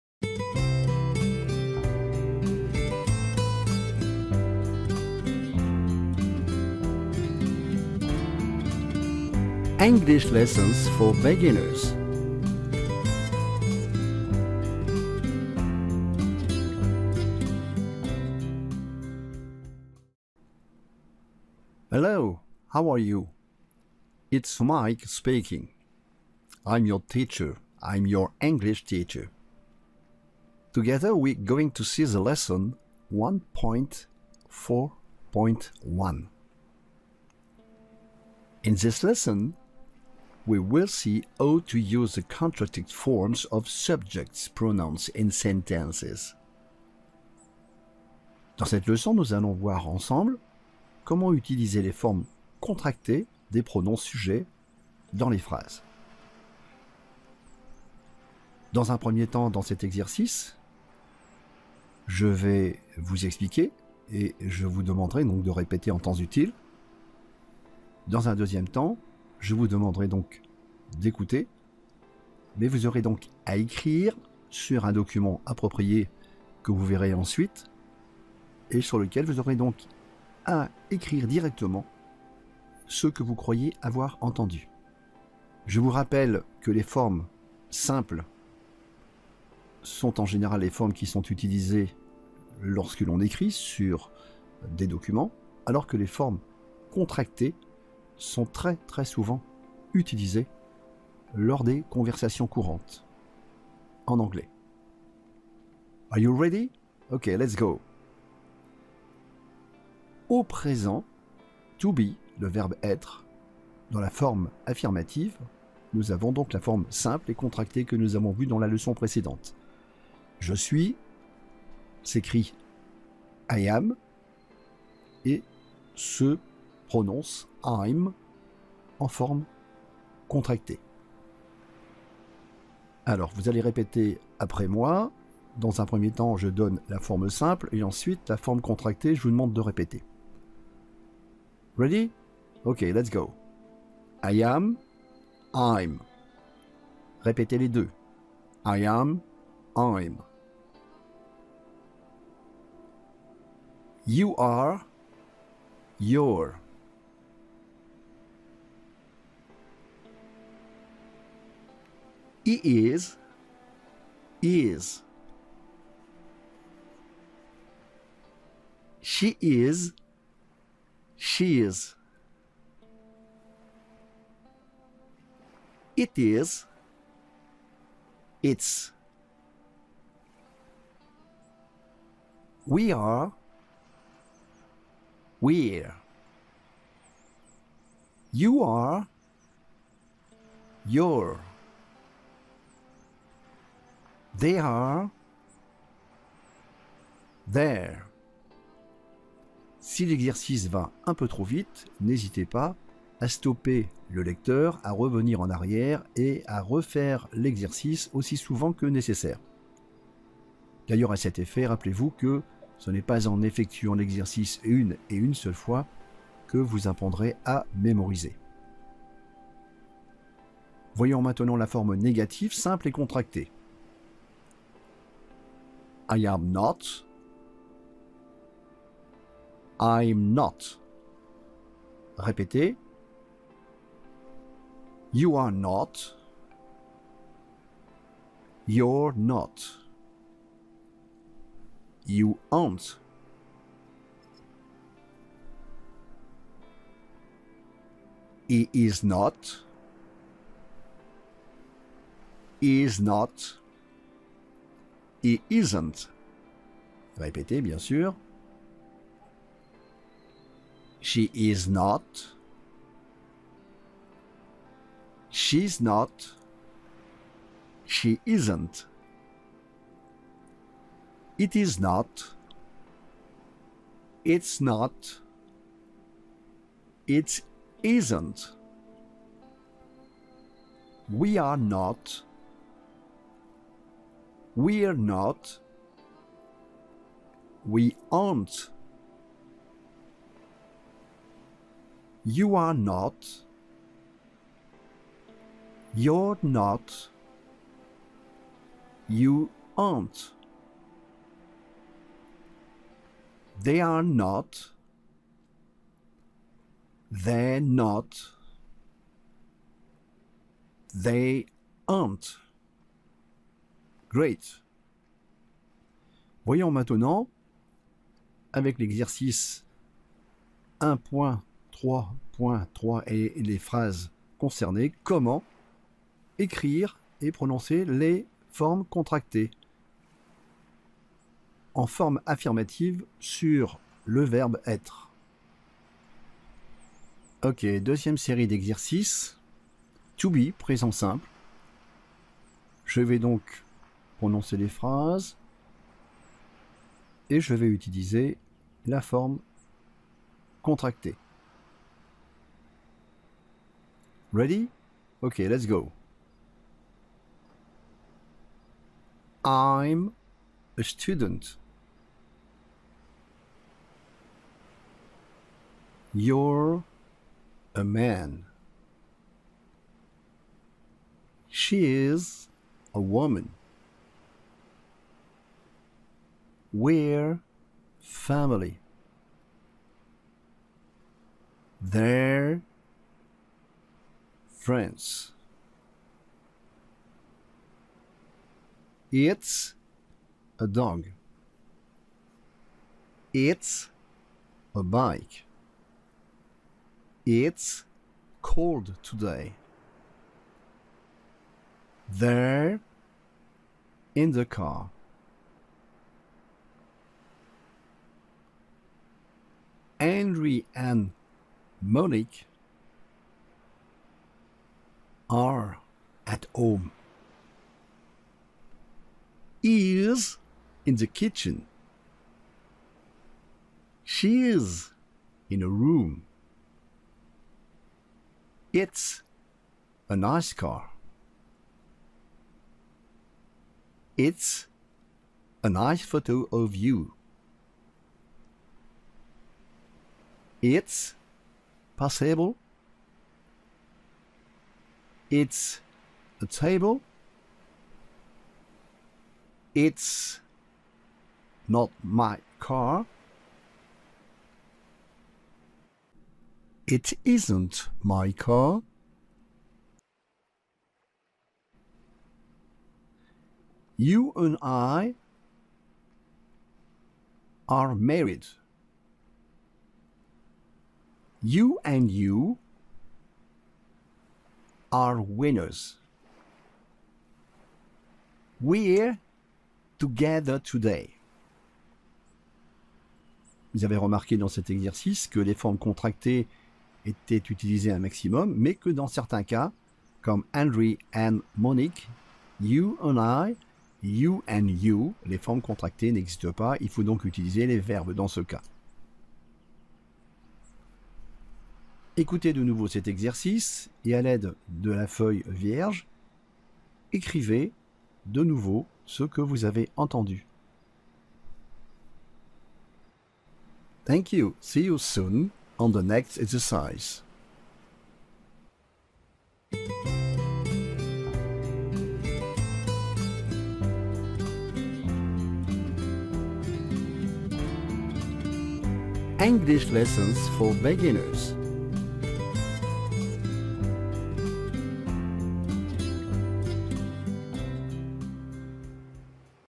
Lesson 0001 4 1x2x3 Exercises Contracted forms To Be
Cette leçon est constituée de 3 exercices audio-écrits orientée sur les formes écrites et parlées en Anglais avec les formes contractées.
Les forms affirmatives et négatives sont étudiées dans la vidéo.
Suite logique de l aleçon 0001.4.0 La Construction des phrases en Anglais.